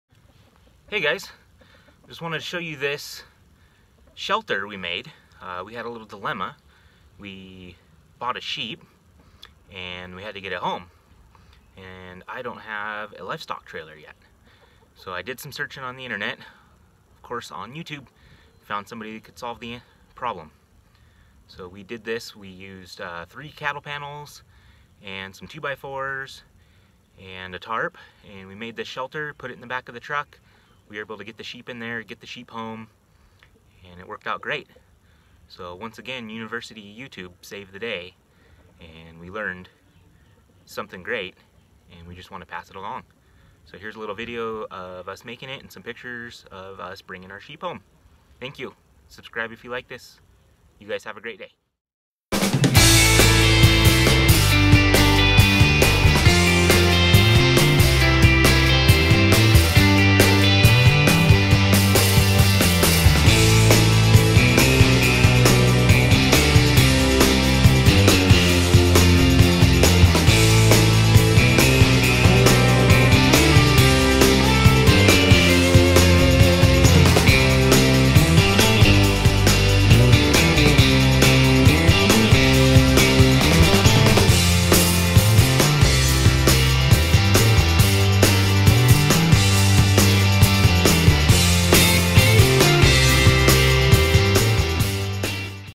Transport sheep, goats, pigs, or other livestock in your truck. DIY livestock transport solution!
We had to build this to get our lamb home.  It was easy to make and worked great!